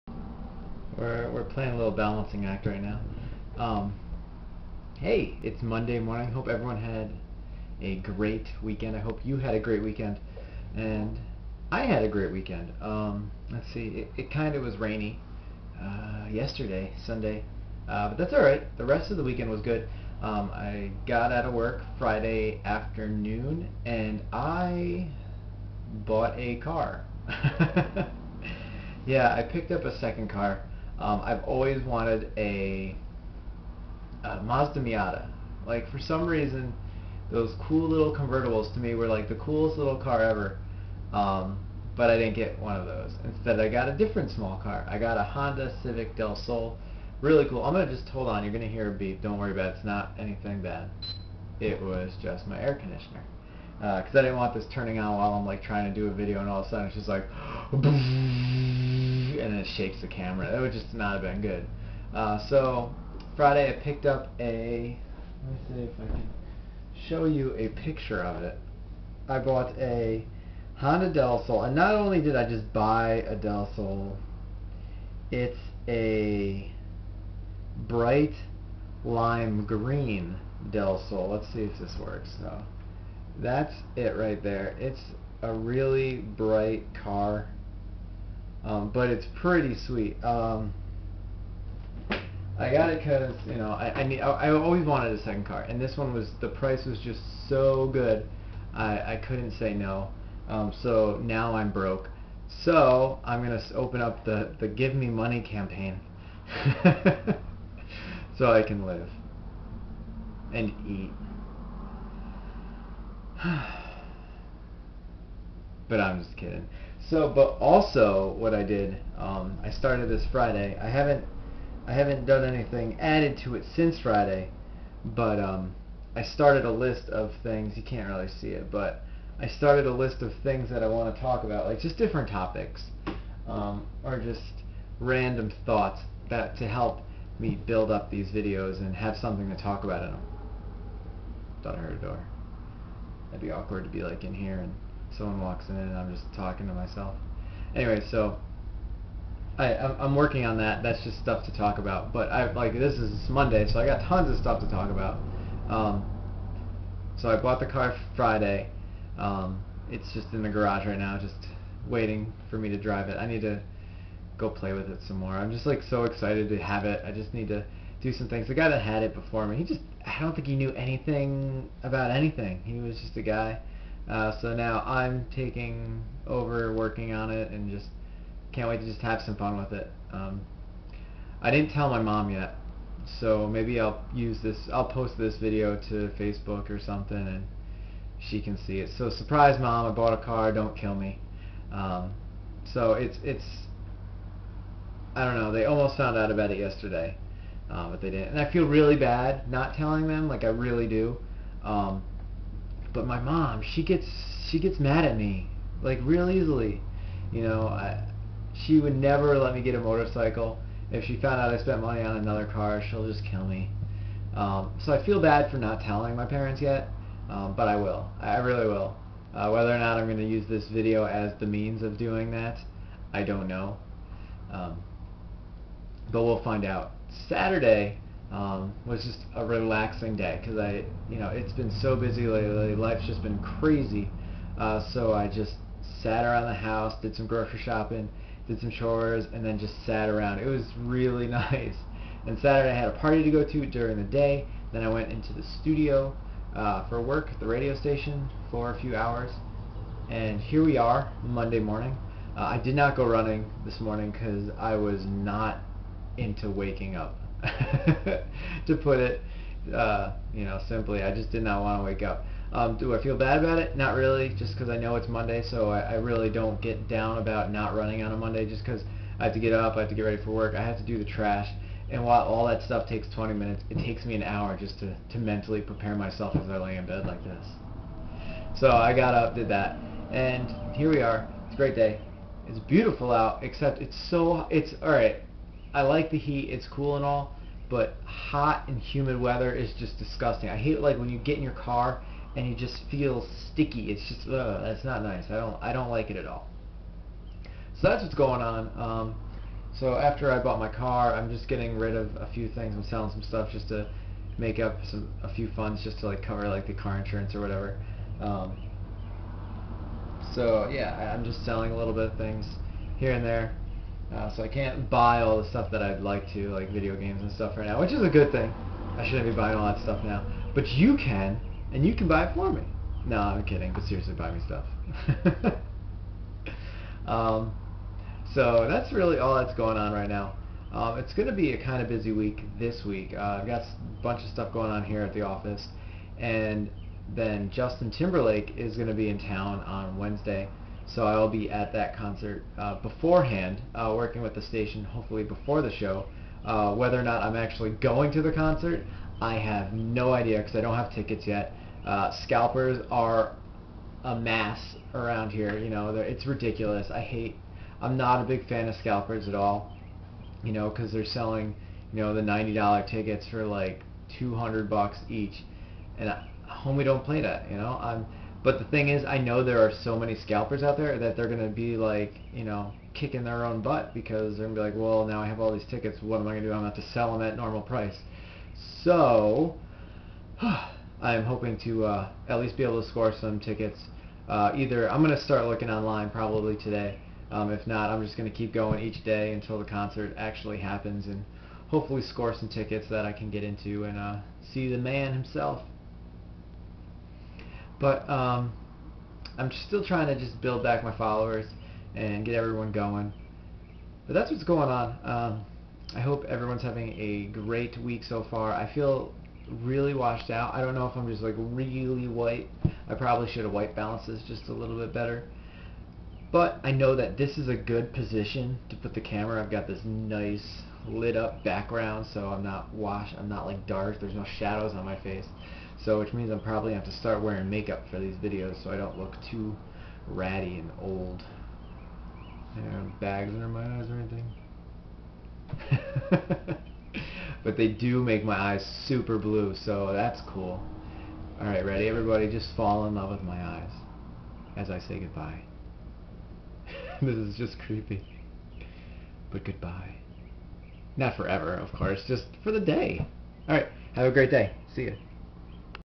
Mink's Mind - July 14th, 2014 - I Bought a Del Sol
So over the weekend, I bought a second car, because we are soon gonna need one. I haven't told my parents... until this video. Surprise mom!